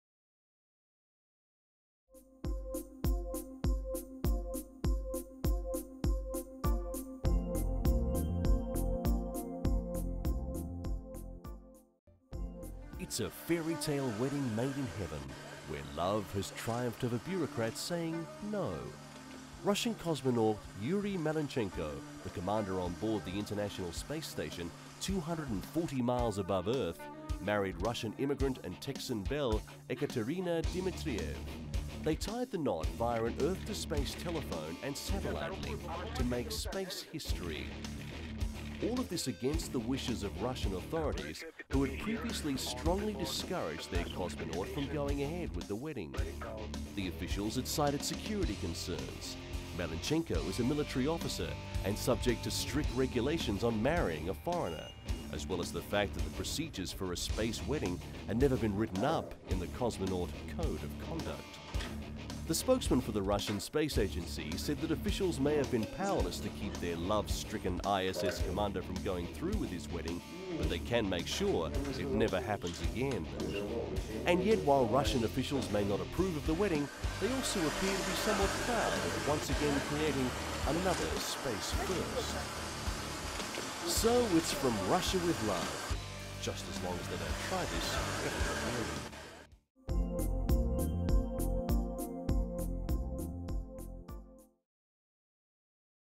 Space Wedding
Moments of Truth: Space Vol. 10

Come along as we take a look at the final frontier, and see how our adventures in space have impacted our daily lives!